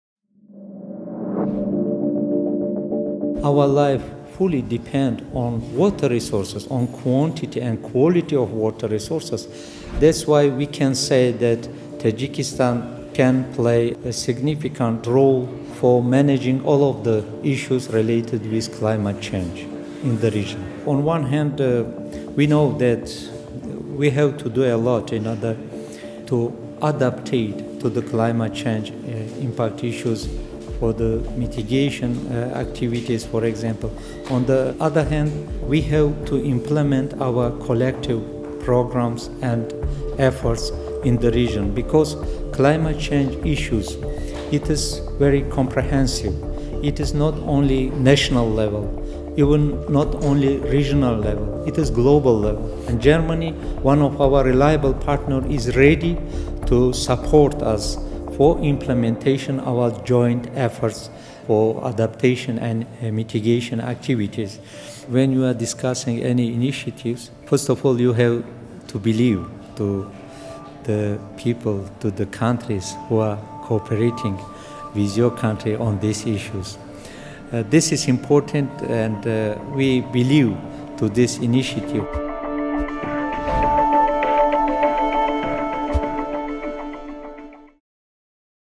Tajikistan’s Foreign Minister Muhriddin at the Green Central Asia Conference
Tajikistan, indeed all of Central Asia, is a hot spot where climate change threatens to result in severe economic, development and environmental losses. On the sidelines of the Green Central Asia conference in Berlin on January 28th, 2020, Foreign Minister of Tajikistan H.E. Sirojiddin Muhriddin gave an interview on his country’s strategic approach to addressing climate impacts and his hopes for the Green Central Asia initiative.

"Our lives fully depend on water resources, on quantity and quality of water resources. That is why we can say that Tajikistan can play a significant role for managing all of the issues related with climate change in the region.

On one hand, we know that we have to do a lot in order to adapt to the climate change impact issues for the mitigation activities, for example. On the other hand, we hope to implement our collective programs and efforts in the region because climate change issues, it is very comprehensive, it is not only on a national level, not only on a regional level. It is on a global level.

And Germany, one of our reliable partners, is ready to support the implementation of our joint efforts for adaptation and mitigation activities. When discussing any initiative, first of all, you have to believe in the people, in the countries, who are cooperating with your country on these issues. This is important. And we believe in this initiative."

The production of this video is supported by the German Federal Foreign Office.

Directed by: Anna Maria Link (adelphi). Produced by: Paul Müller-Hahl (Lichtbilder)